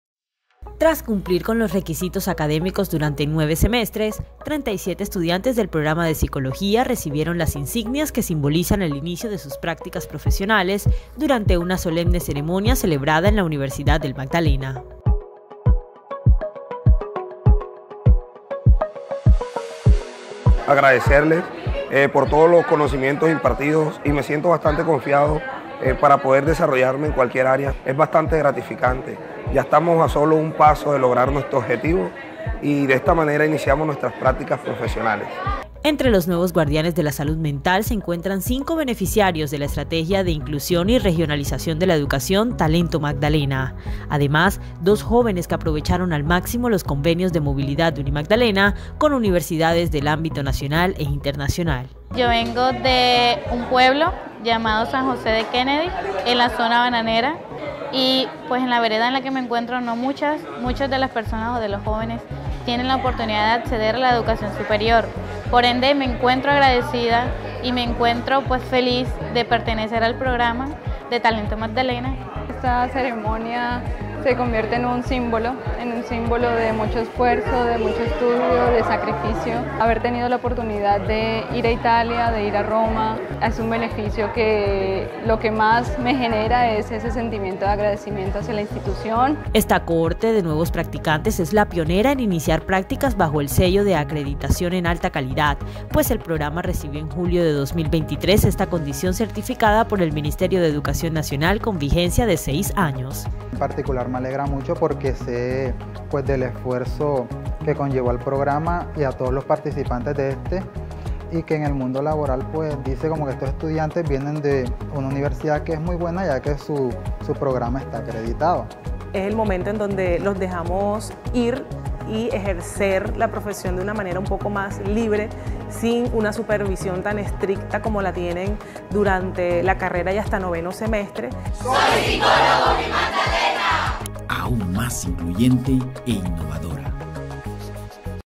Estudiantes de Psicología UNIMAGDALENA reciben aval como guardianes de la salud mental
Con la simbólica ceremonia de entrega de insignias, un grupo de 37 estudiantes del Programa de Psicología de la Universidad del Magdalena culminó satisfactoriamente nueve semestres de formación académica e iniciará la etapa de prácticas profesionales. Directivos y docentes de la unidad académica les otorgaron a los futuros psicólogos el aval para su primer ejercicio profesional sin supervisión permanente, el cual se realizará a lo largo del próximo semestre.